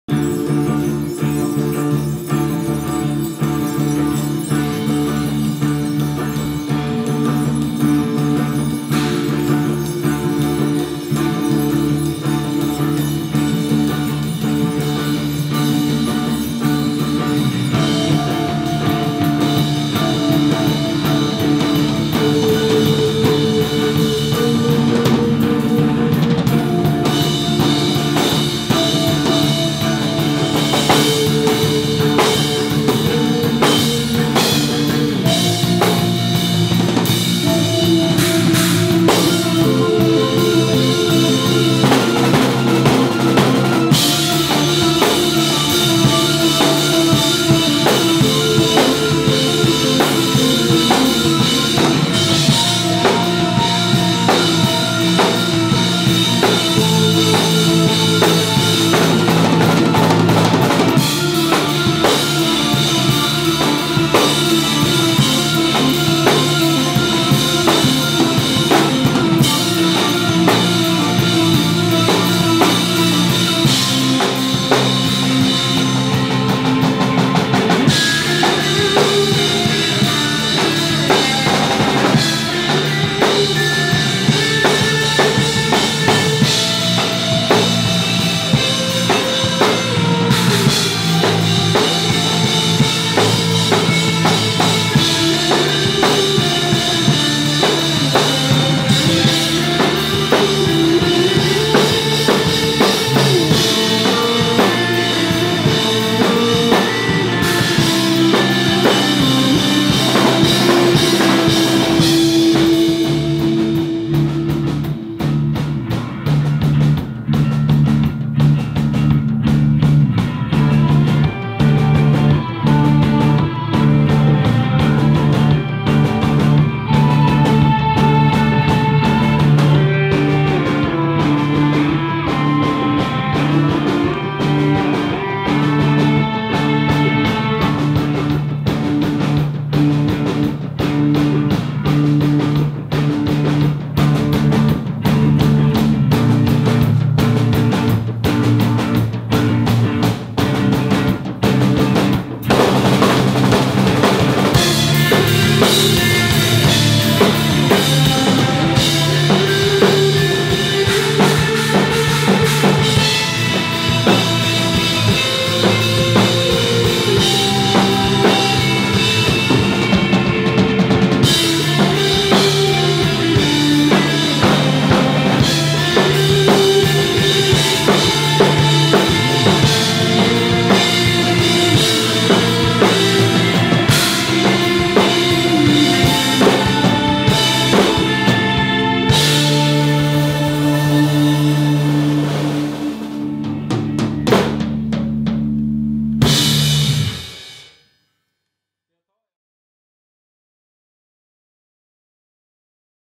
Pixie Ninja rehearsal @ Roth-Händle Studios
Pixie Ninja is rehearsing with Mattias Olsson, and Fredrik Klingwall at Roth-Händle Studios (May 11-13/2018), for the  upcoming festival; Close To The Rain-Bergen progfestival (June 9th/ 2018).

Filmed and edited by: Martin Gustafsson ©